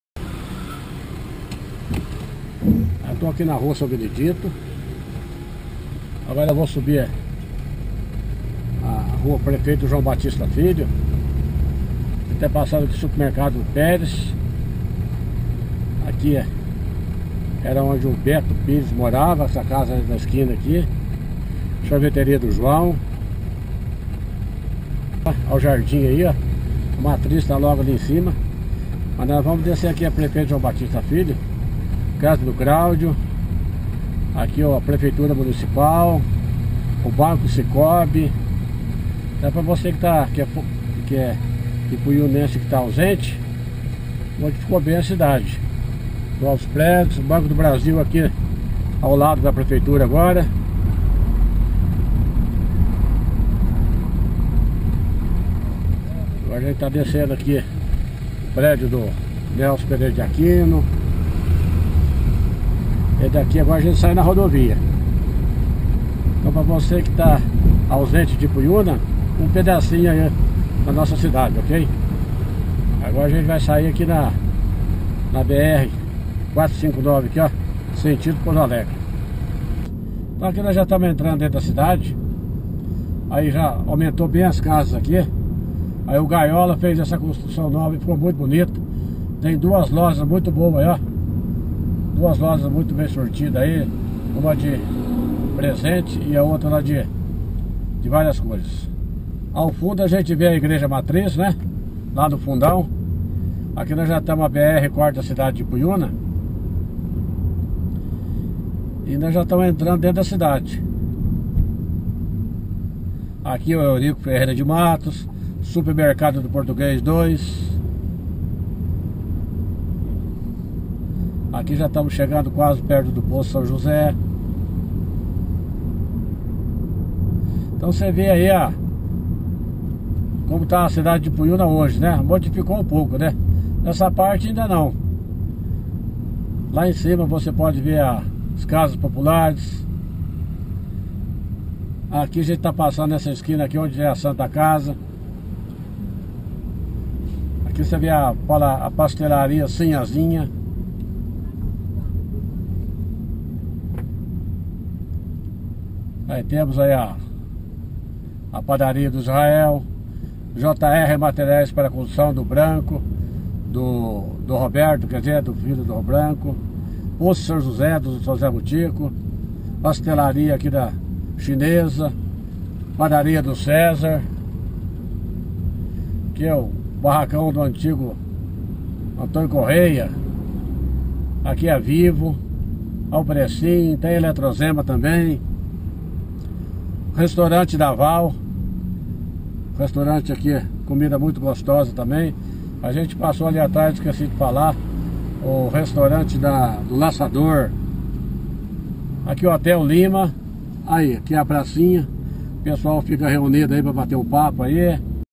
Passeio por Ipuiuna parte 1
Imagens gravadas por Branco Procópio com uma GRO PRO de dentro de um carro mostrando um pouco de Ipuiuna.